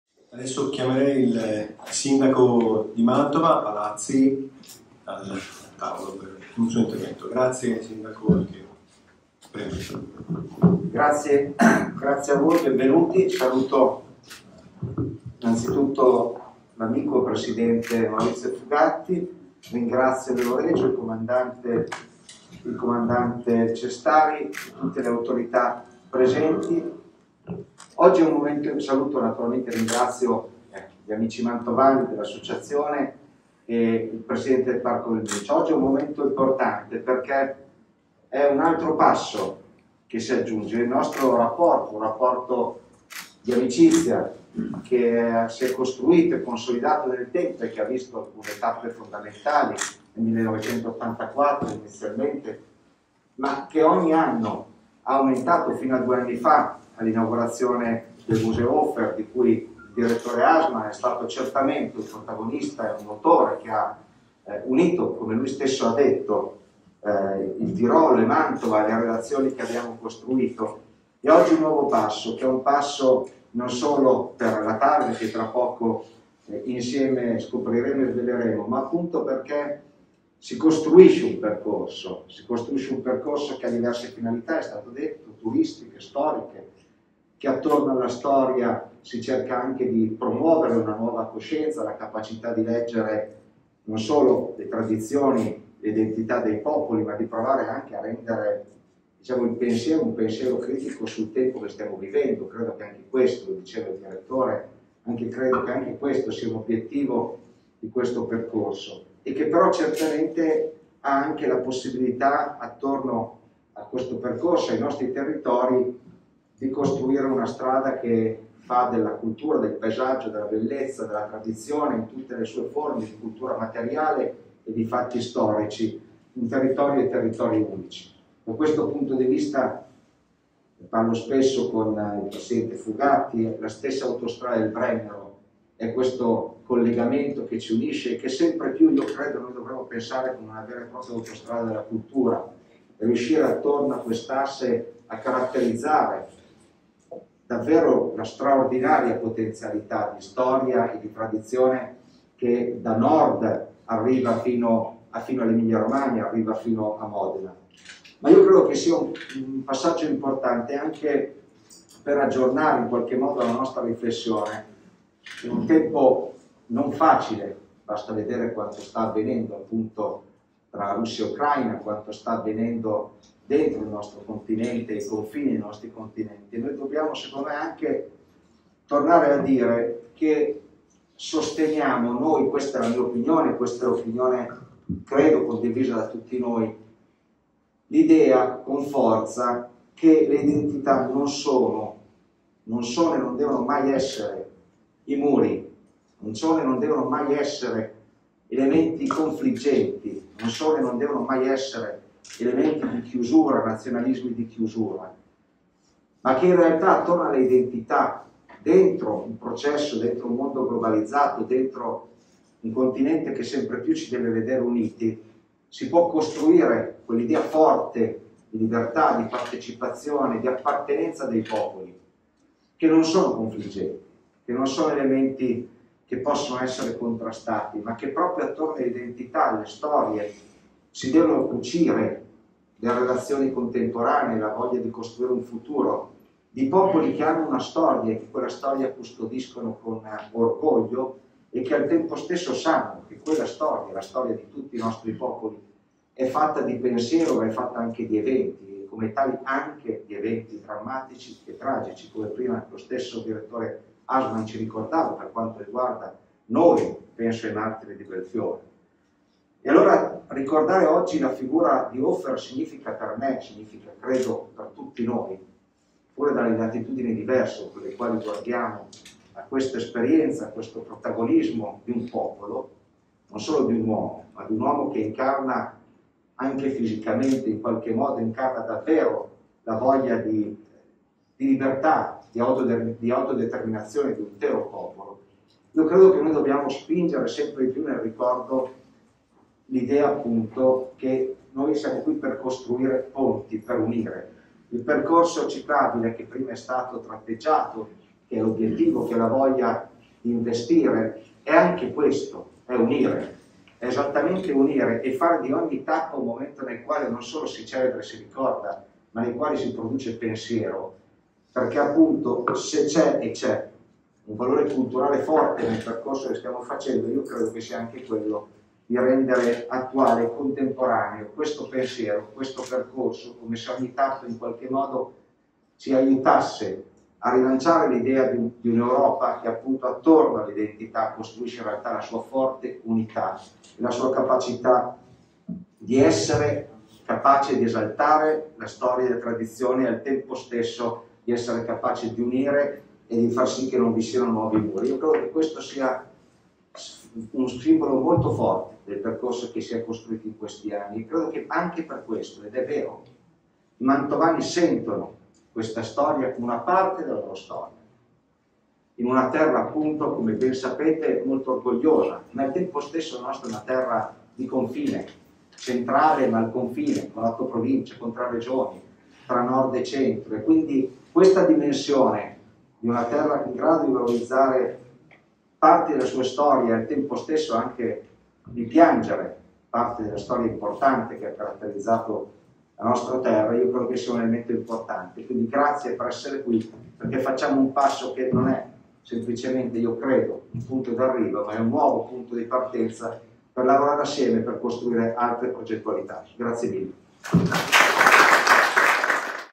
INTERVENTO DI MATTIA PALAZZI, Sindaco di Mantova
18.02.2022 - CONFERENZA STAMPA - SALA CONVEGNI PARCO DEL MINCIO - MANTOVA
I LUOGHI DI ANDREAS HOFER: L’IMPORTANZA DELLA MEMORIA, L’OPPORTUNITÀ PER I TERRITORI POSIZIONAMENTO DELLA TARGA DELL’ITINERARIO “HOFER ERA QUI“
Hanno partecipato:
• Mattia Palazzi , Sindaco di Mantova
• Enzo Cestari, Comandante della Federazione Schützen del Welschtirol
• Giuseppe Ferrandi, Direttore della Fondazione Museo storico del Trentino
• Paolo Predella, Presidente Associazione Porta Giulia Hofer
• Maurizio Pellizzer, Presidente del Parco del Mincio
• Peter Assmann, Direttore dei Musei del Tirolo
• Carlo Bottani, Presidente della Provincia di Mantova
• Maurizio Fugatti, Presidente del GECT Euregio Tirolo-Alto Adige-Trentino e Presidente della Provincia autonoma di Trento
•  moderatore Angelo Conte